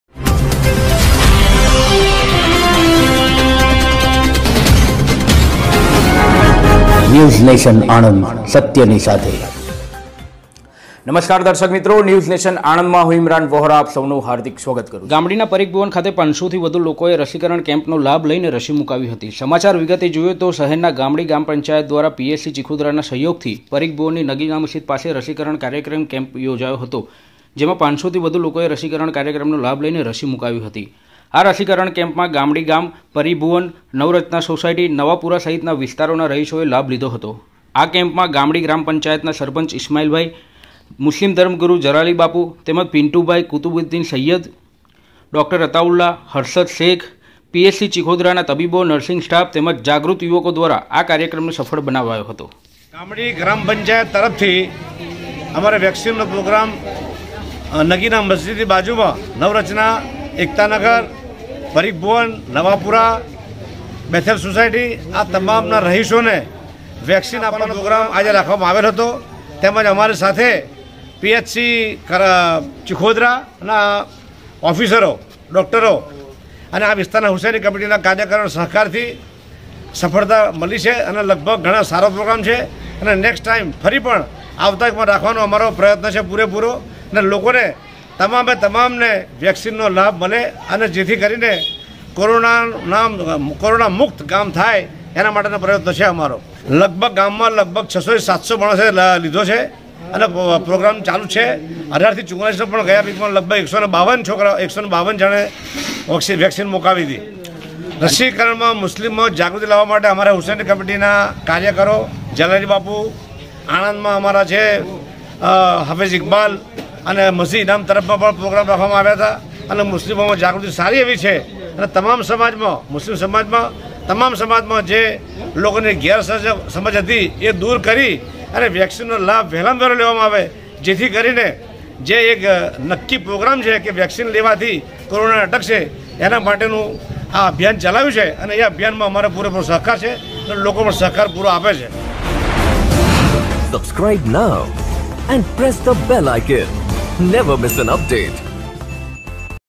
ANAND : શહેરના ગામડી પરીખભુવન ખાતે 500થી વધુ લોકોએ રસીકરણનો લાભ લીધો...
•  આણંદ શહેરના ગામડી ગામ પંચાયત ધ્વારા તેમજ પીએચસી ચિખોદરાના સહયોગથી પરીખભુવનની નગીના મસ્જિદ પાસે રસીકરણ કાર્યક્ર્મ યોજાયો..

• 500 થી વધુ લોકોએ આ રસીકરણ કાર્યક્ર્મ નો લાભ લઈને રસી મુકાવી...

• રસીકરણ કેમ્પમાં ગામડી ગામ,પરીખભુવન નવરચના સોસાયટી, નવપુરા સહિતના વિસ્તારોના નાં રહીશોએ લાભ લીધો હતો.

• ગામડી ગ્રામ પંચાયત સરપંચ ઇસ્માઇલભાઈ આદમ ભાઈ, મુસ્લિમ ધર્મગુરુ જલાલી બાપુ તેમજ પીન્ટુભાઇ, કુતુબ સાઈયેદ, ર્ડો અતાલ્લા, હર્શદ શેઇક phc ચિખોદરાના તબીબો નર્સિંગ સ્ટાફ તેમજ જાગૃત નવયુવાકો ધ્વારા આ કાર્યક્રમને સફળ બનાવ્યો હતો..

•  આપની આસપાસ ની બનતી ઘટનાની સત્ય માહિતી પ્રકાશિત કરવા માટે સંપર્ક કરો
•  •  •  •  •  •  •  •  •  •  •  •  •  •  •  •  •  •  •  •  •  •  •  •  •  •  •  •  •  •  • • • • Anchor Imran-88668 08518